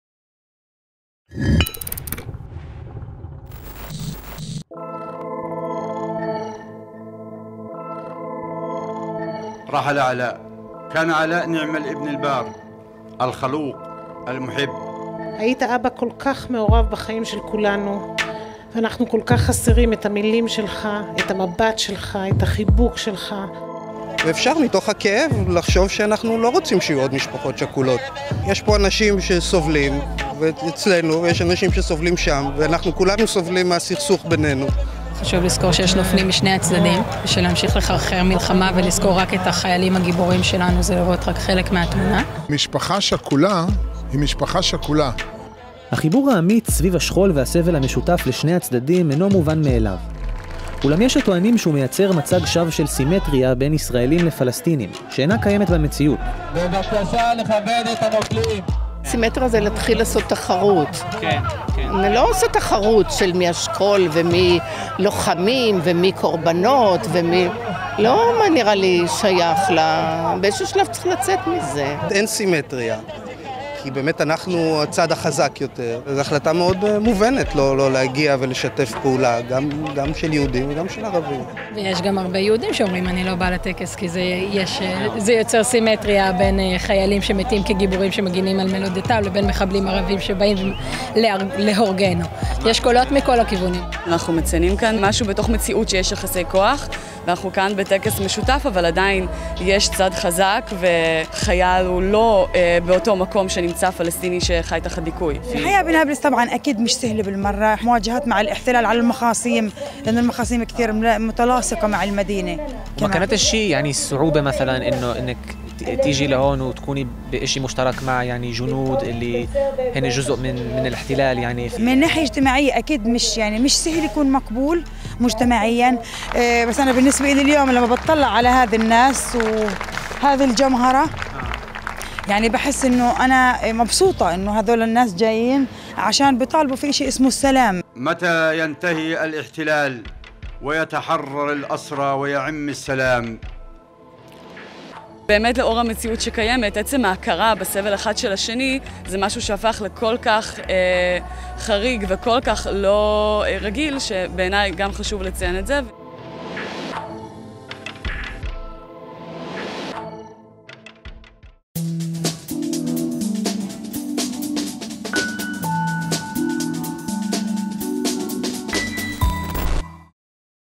Joint Bereavement | Asymmetrical Relations
Does a joint Israeli-Palestinian memorial ceremony commemorating the victims of both sides creates symmetry of bereavement? Is "a bereaved family a bereaved family" as one of the speakers in the article told us, or does the ceremony, which exists as long as one side is the occupier and the other side struggles to free itself, presents a false state of symmetry?